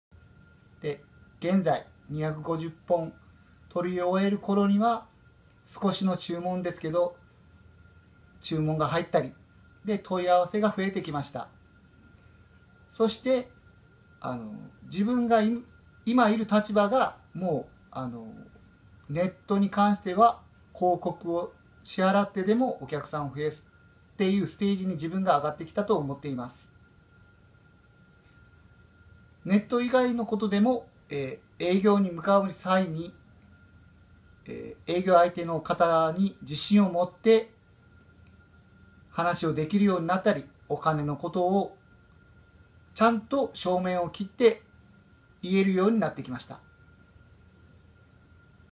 ２５０ 成功への６次産業化 対馬
６次産業化の成功は「独立自尊」
ひとり、一人が強い農水就業者であり経営者でなければ
ならない。先ずは自ら鍛える事が６次化の優位性を生むことになる。
ここに紹介する人材は離島・へき地に住む農水就業者である。彼らはICT/SNSを利活用する事によって自ら能力を一歩高め、強い農水就業者になった。